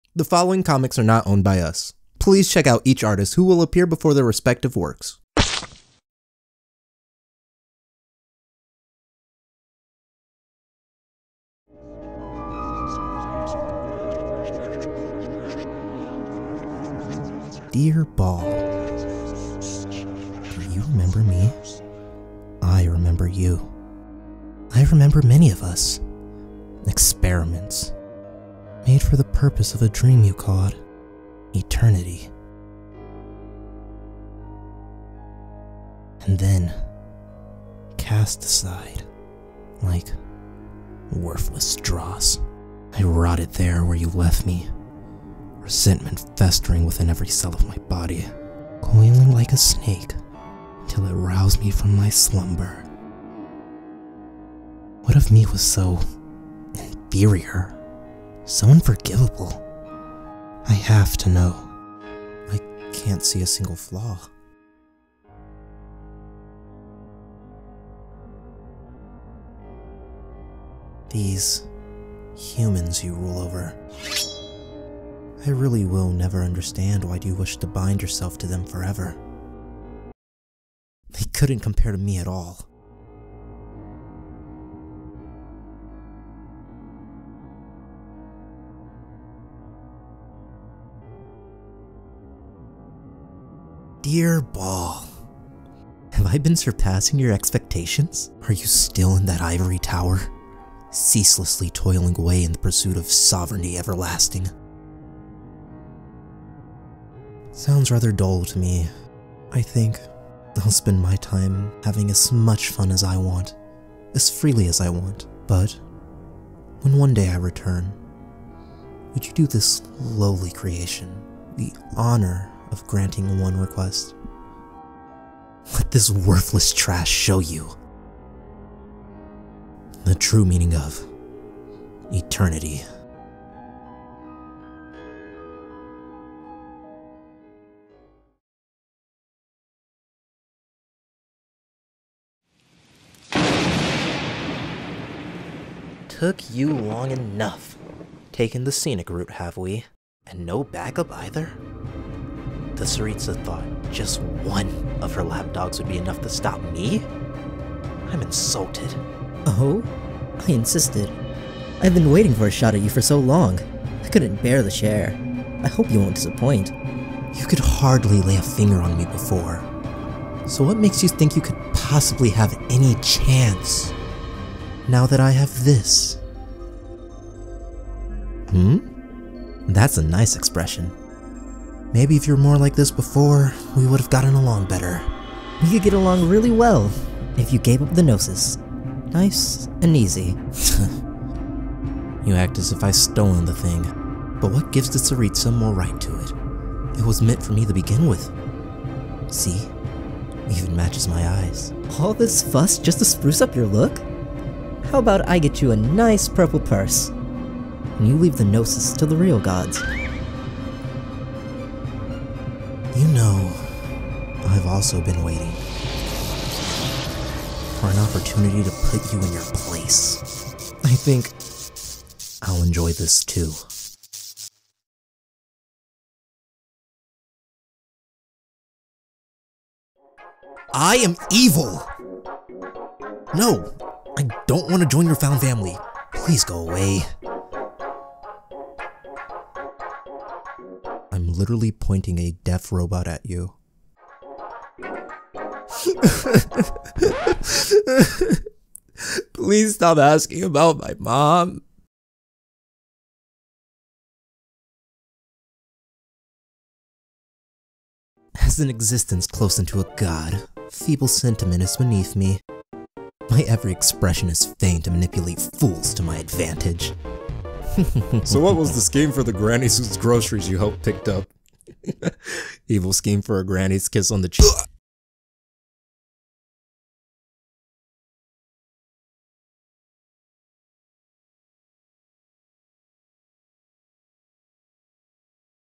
Wanderer Takes Back the Gnosis | Genshin Impact Compilation
Artists In Order:
Comics In Order:

A new compilation featuring the wanderer or scaramouche from Genshin Impact with the appearance of Childe / Tartaglia / Ajax
Like the dubs? Follow or at least go and like the ORIGINAL Creator and leave us a sub while you're at it. We couldn't of done this without either of you.